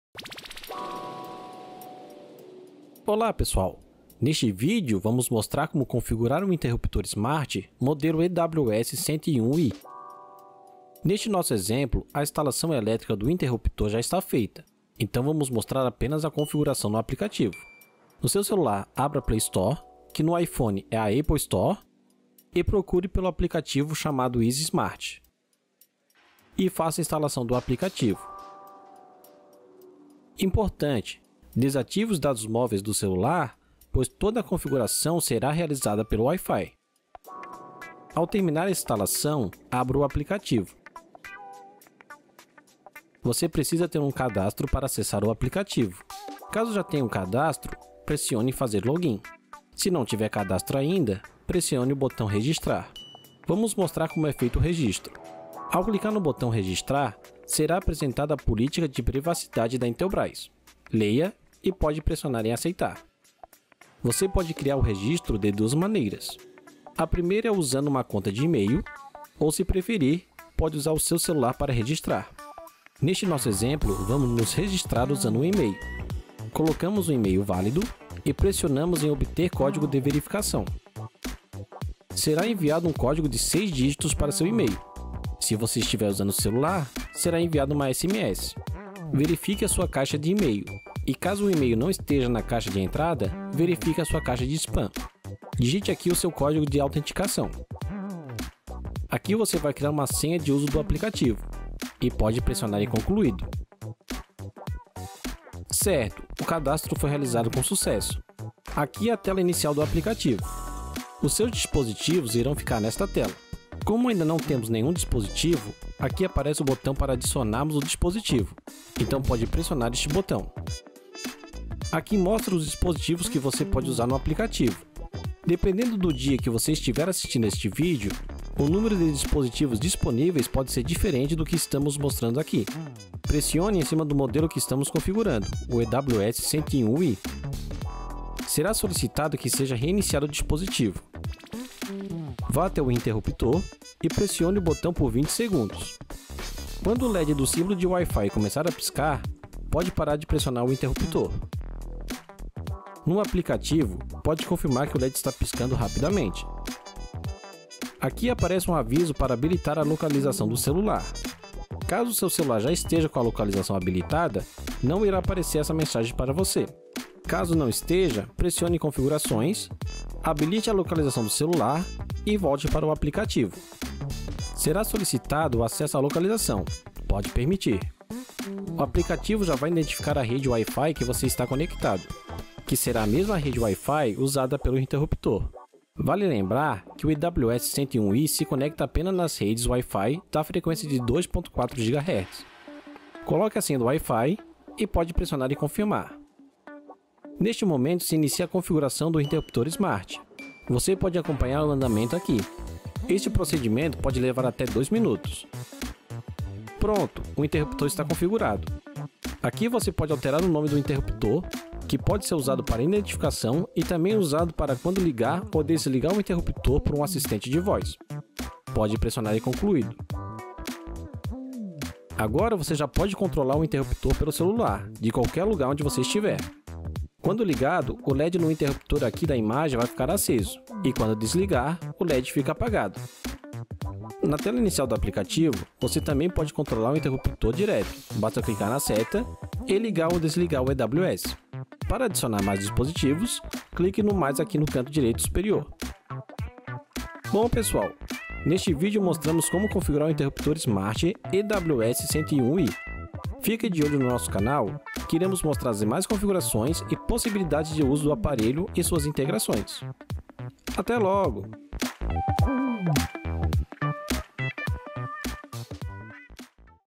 Configurando o interruptor smart EWS 101 I - i1850
Neste vídeo vamos mostrar como realizar a configuração do interruptor smart modelo EWS 101 I da Intelbras.

O interruptor inteligente EWS 101 I de embutir em caixa 4×2” (caixa de interruptor) é utilizado no acionamento automático através de aplicativo ou manual através da tecla de lâmpadas em ambientes residenciais e comerciais. O EWS 101 I permite que o usuário acione, controle e automatize remotamente o interruptor através do aplicativo IZY Smart de maneira fácil e inteligente. O produto alia características de praticidade, economia de energia e segurança.